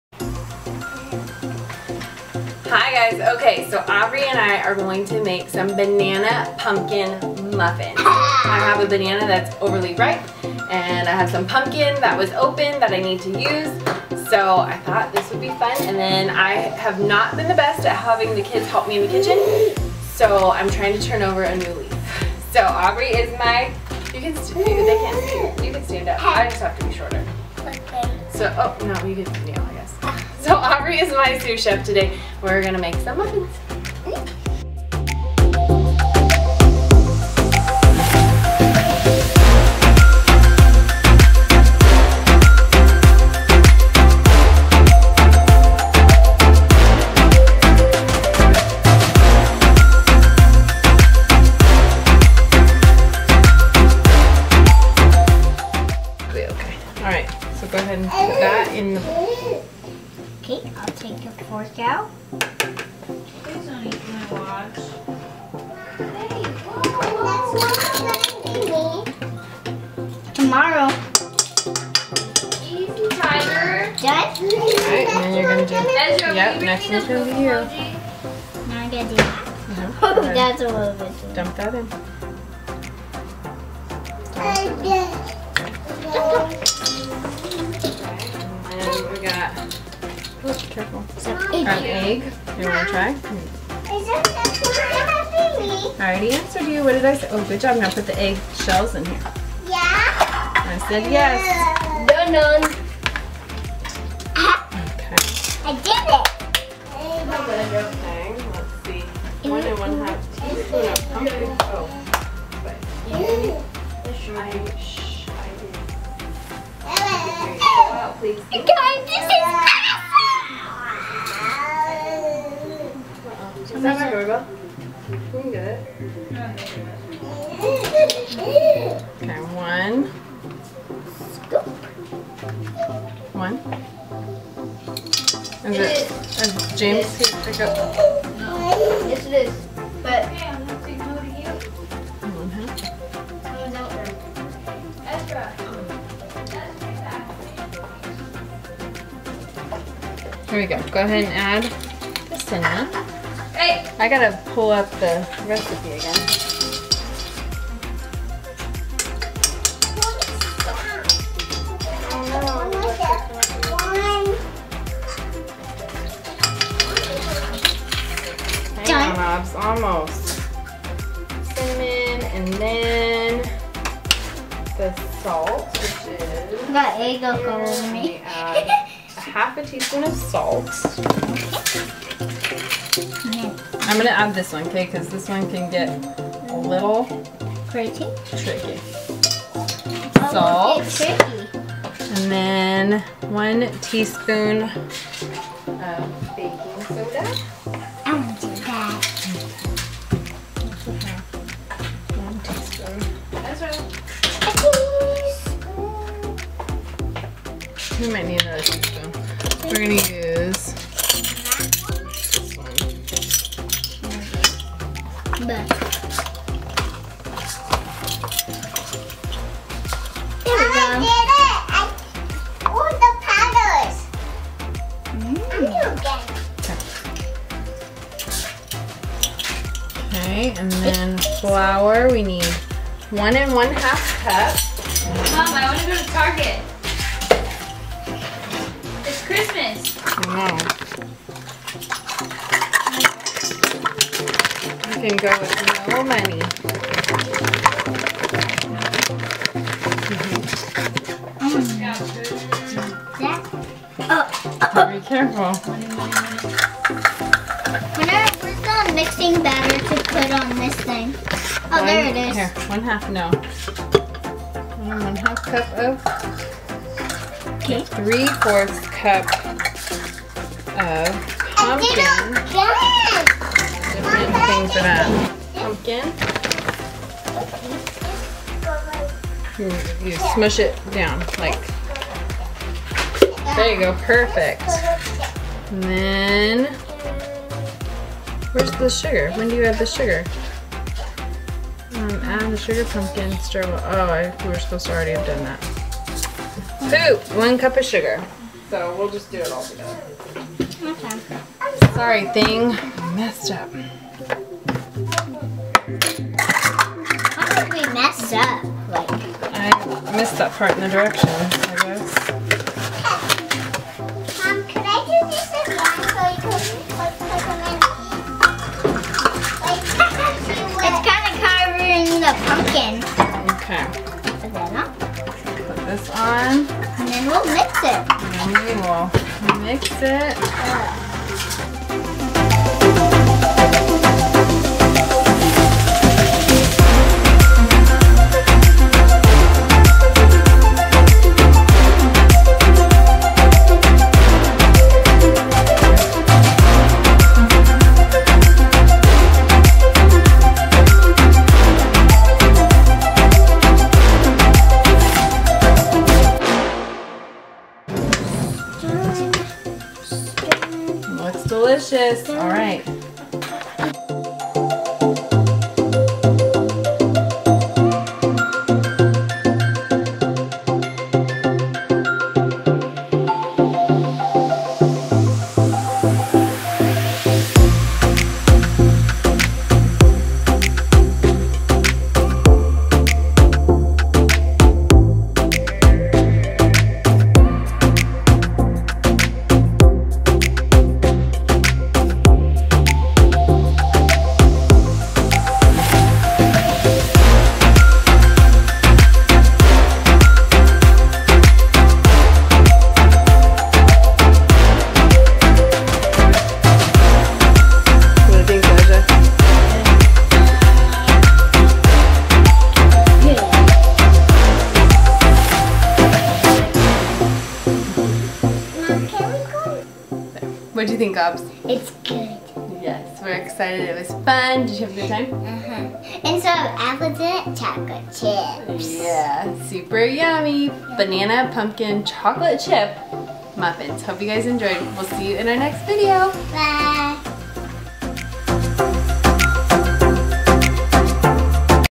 BAKING GLUTEN FREE PUMPKIN BANANA CHOCOLATE CHIP MUFFINS
AUBRIE AND I BAKED SOME AMAZING MUFFINS WE FOUND ON PINTREST.

HERE ARE THE INGREDIENTS WE USED

Ingredients
Vegetarian

Produce
2 Bananas
3/4 cup Pumpkin, canned
Refrigerated
1 Egg

Baking & Spices
1 tsp Cinnamon
1 1/2 cups Flour ( WE USED ALMOND FLOUR)
1 1/2 tsp Pumpkin spice
1 cup Sugar
1 Tea baking soda
1/2 Tea salt
1 tsp Vanilla

Dairy
1/4 cup Butter